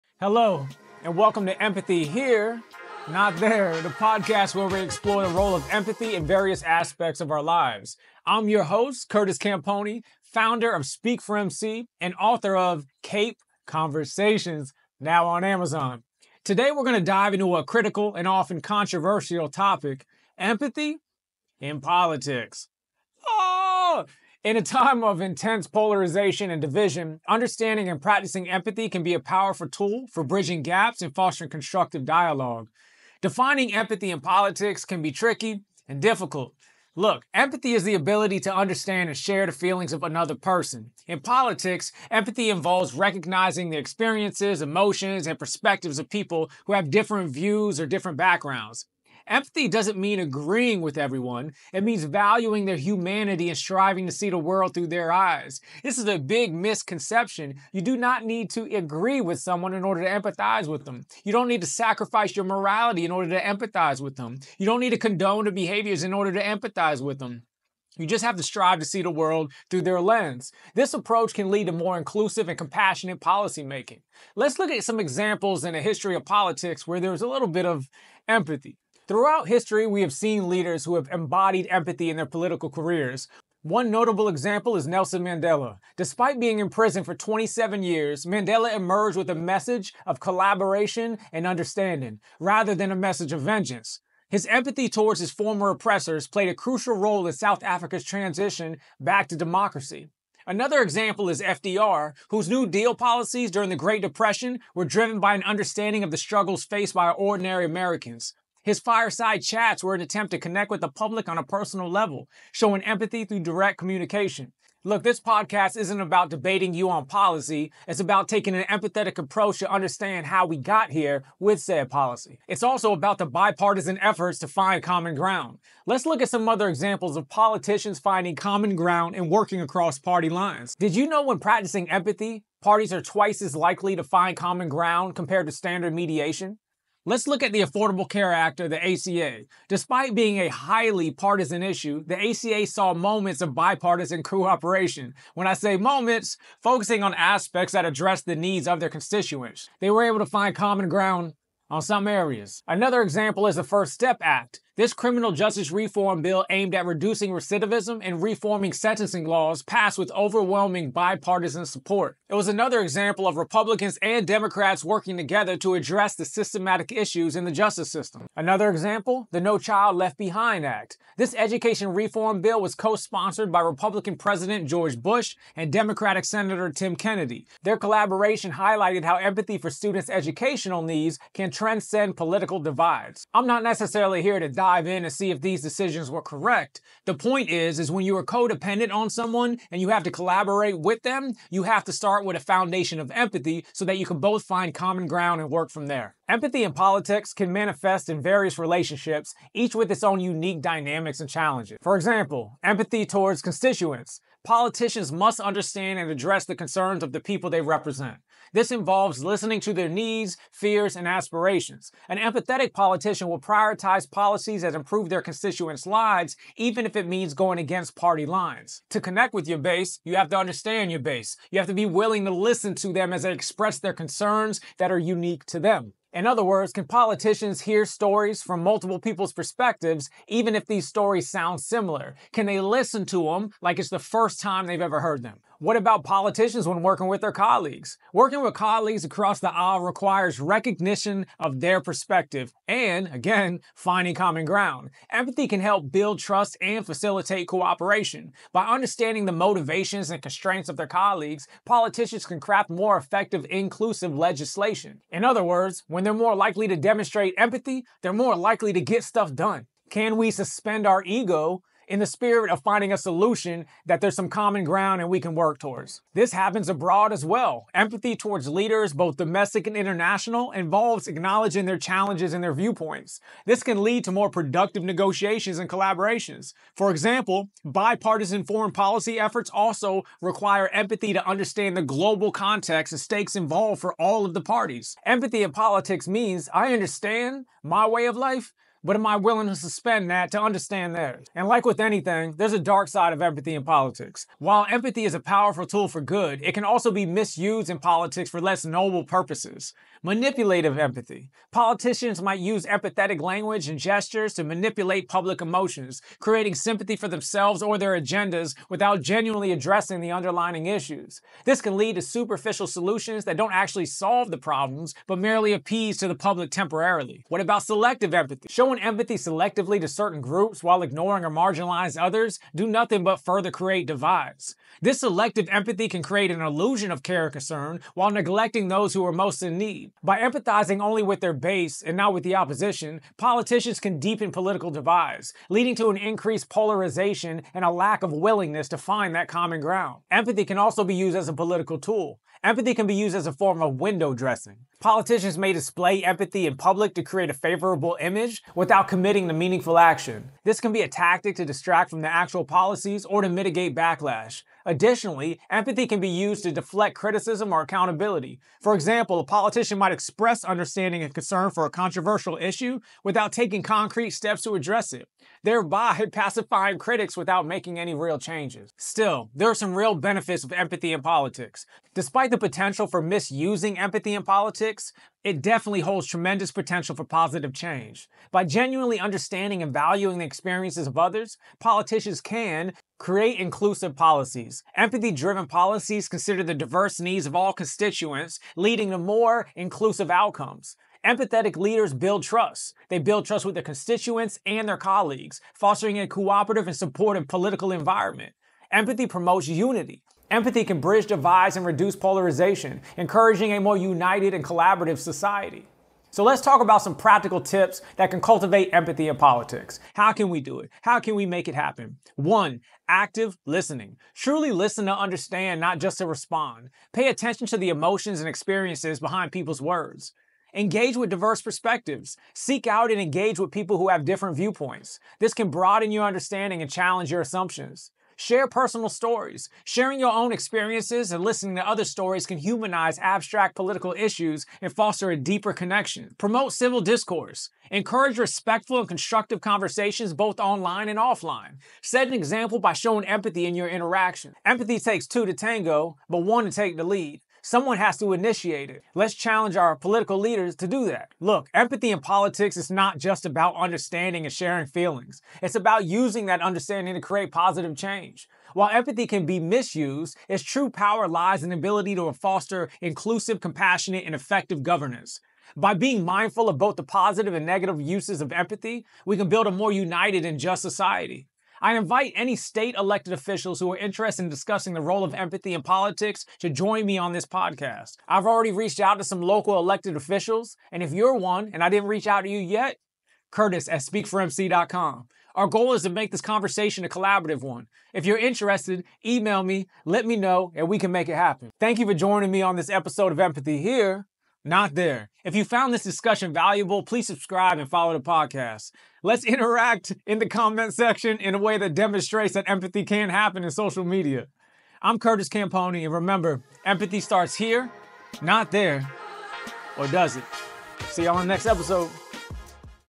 Episode 2 | Empathy in Politics: Bridging Divides and Finding Common Ground
In this episode of "Empathy Here, Not There," host Curtis Campogni dives into the crucial and often controversial topic of empathy in politics. As polarization and division seem to dominate the political landscape, Curtis explores how empathy can be a powerful tool for bridging gaps and fostering constructive dialogue.

Curtis defines empathy in the political context, emphasizing that it does not mean agreeing with everyone but rather valuing humanity and striving to see the world through others' eyes. He highlights historical examples of empathetic leadership, such as Nelson Mandela and Franklin D. Roosevelt, and discusses bipartisan efforts like the Affordable Care Act, the First Step Act, and the No Child Left Behind Act.

The episode also addresses the potential dark sides of empathy in politics, such as manipulative empathy, selective empathy, and empathy as a political tool. Curtis provides practical tips for cultivating empathy in politics, including active listening, engaging with diverse perspectives, sharing personal stories, and promoting civil discourse.

Join Curtis as he challenges political leaders to take the lead in demonstrating empathy and invites state-elected officials to discuss the role of empathy in politics on the podcast.

Tune in to learn how empathy can foster inclusive, compassionate, and effective governance, and help build a more united and just society.

Remember, "Empathy starts here, not there... or does it?"

Key Points:
Introduction to empathy in politics
Historical examples of empathetic leadership
Bipartisan cooperation in politics
Potential misuse of empathy in politics
Practical tips for cultivating empathy in politics
Call to action for elected officials to join the discussion

Speak4MC.com